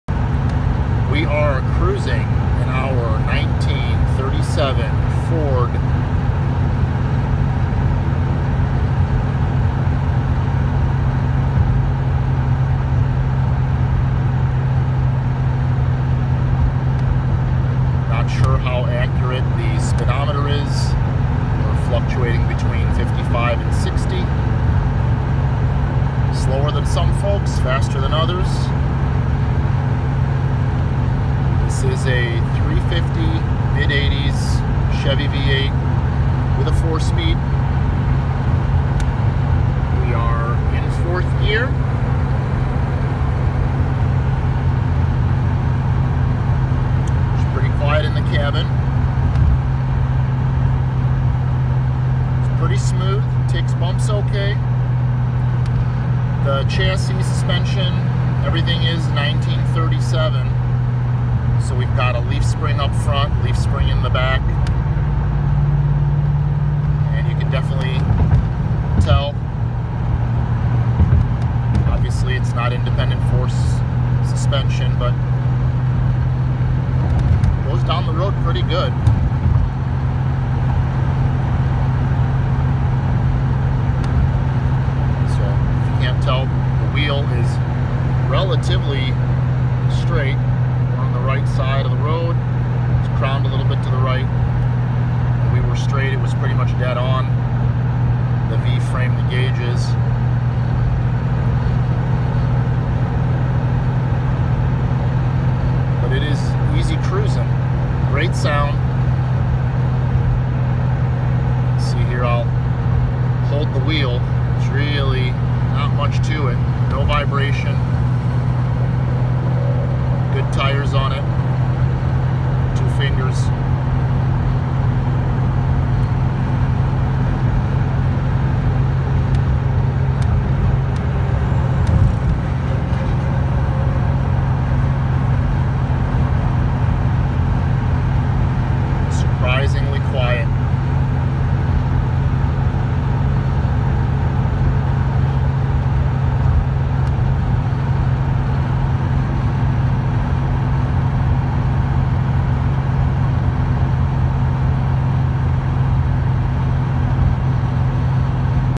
37' Ford Restomod Highway Cruising
We cruise down the highway in the 37' Ford Restomod listening to the engine and exhaust.